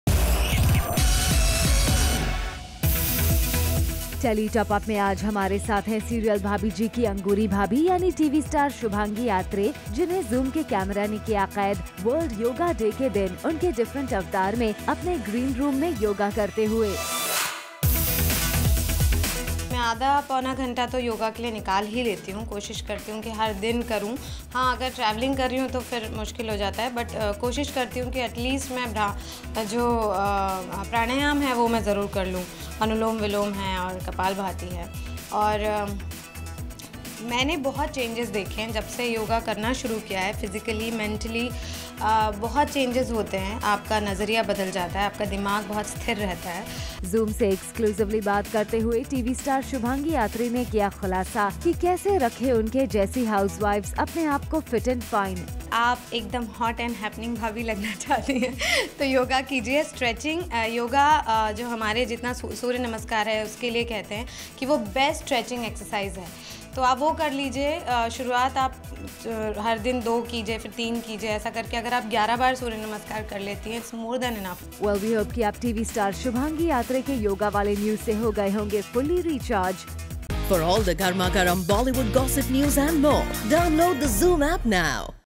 Shubhangi Atre AKA Angoori Bhabi’s Reveals Her Fitness Mantra | International Yoga Day
► Shubhangi Atre AKA Angoori does Yoga on International Yoga Day.